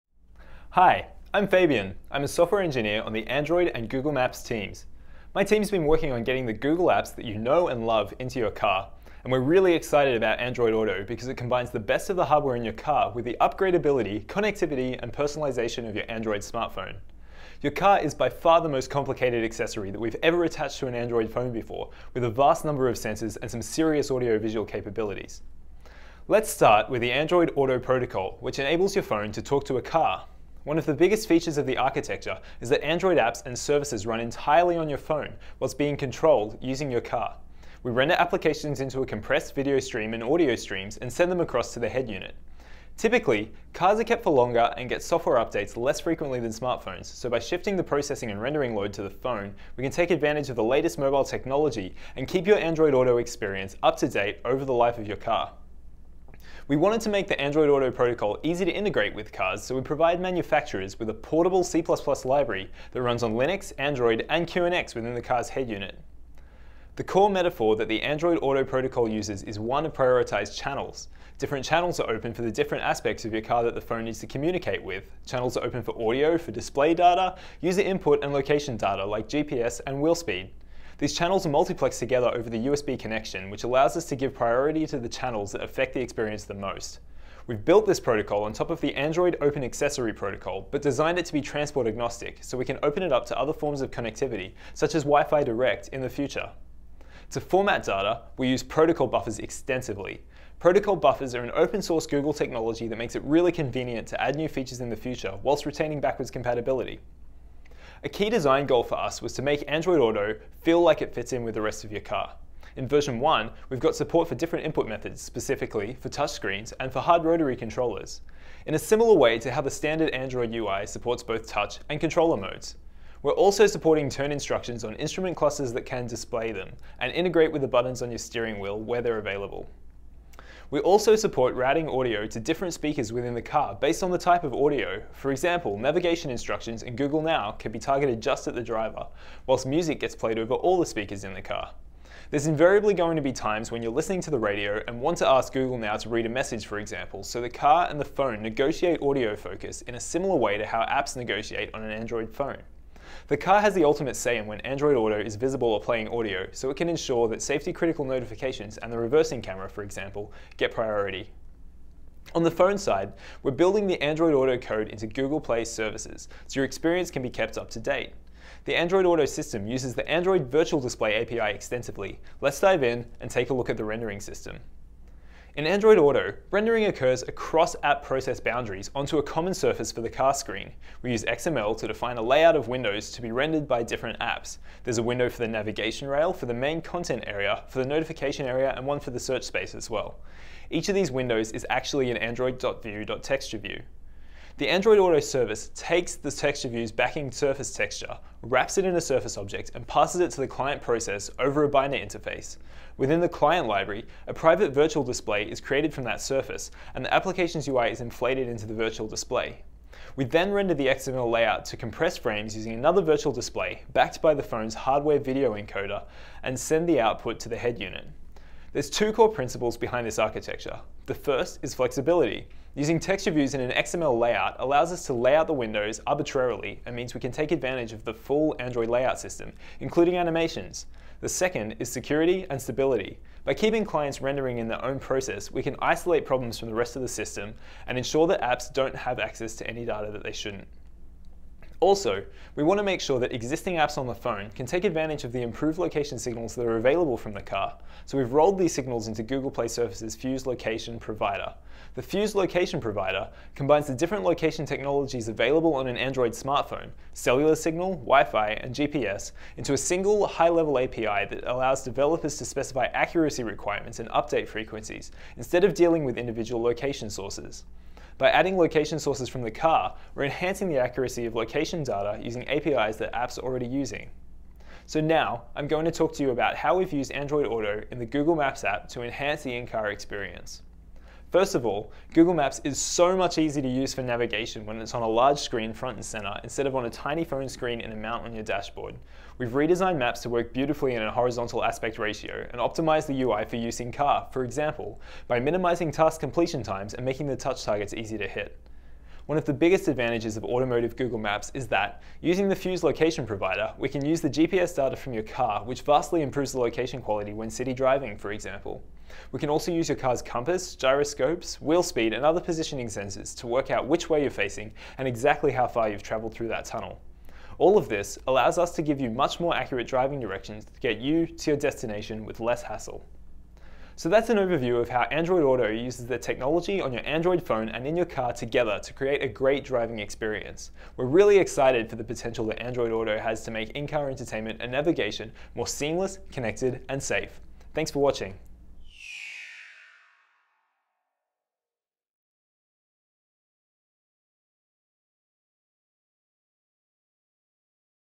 Under the Hood of Android Auto
Your car contains some serious technology. Learn about the Android Auto architecture, which enables you to control Android apps and services running on your phone through your car. In this talk, we'll highlight the key characteristics of the Android Auto protocol, which enables your Android phone to talk to compatible cars. We'll also take a look at some of the details of the Android Auto rendering subsystem, which uses Binders and the Android VirtualDisplay API to composite UI from multiple Android apps. Finally, we'll discuss how we're enhancing the Google Maps app by integrating with the Android Auto platform.